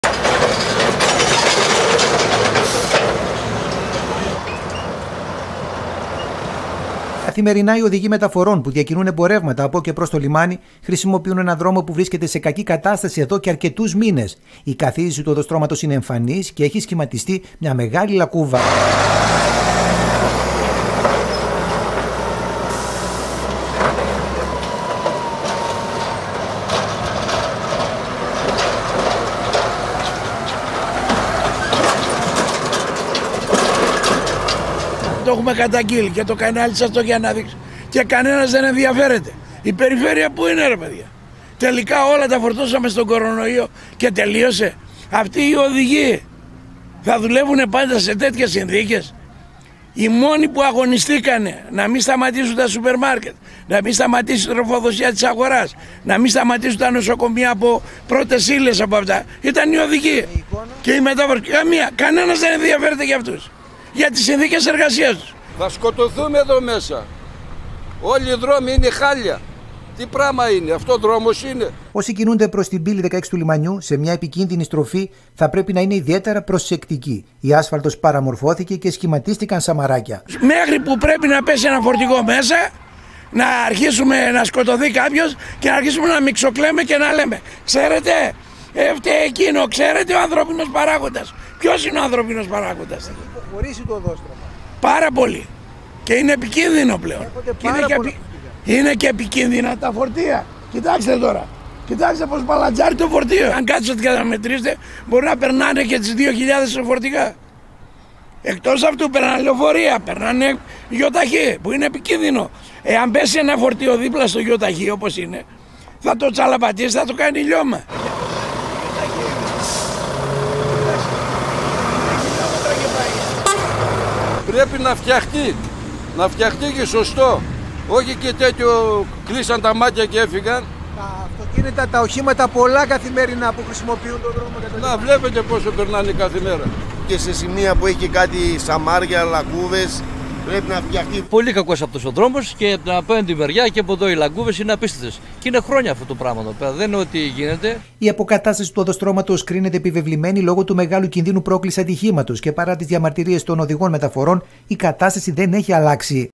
ΑΓΑΝΑΚΤΙΣΜΕΝΟΙ ΕΙΝΑΙ ΟΙ ΟΔΗΓΟΙ ΜΕΤΑΦΟΡΩΝ
Την κακή κατάσταση της οδού, που χρησιμοποιούν καθημερινά χιλιάδες οδηγοί για να διακινήσουν εμπορεύματα από και προς το λιμάνι της Θεσσαλονίκης, έχουμε καταγράψει ουκ ολίγες φορές με την κάμερα του ΑΤΛΑΣ. Η αποκατάσταση του οδοστρώματος ειδικά τους τελευταίους μήνες κρίνεται επιβεβλημένη εξαιτίας του σοβαρού κινδύνου πρόκλησης ατυχήματος. Συγκεκριμένα τα οχήματα πέφτουν μέσα σε μεγάλη λακκούβα, η οποία έχει σχηματιστεί μπροστά στο σηματοδότη της διασταύρωσης που οδηγεί προς την εθνική οδό Θεσσαλονίκης-Αθηνών και το κέντρο της πόλης.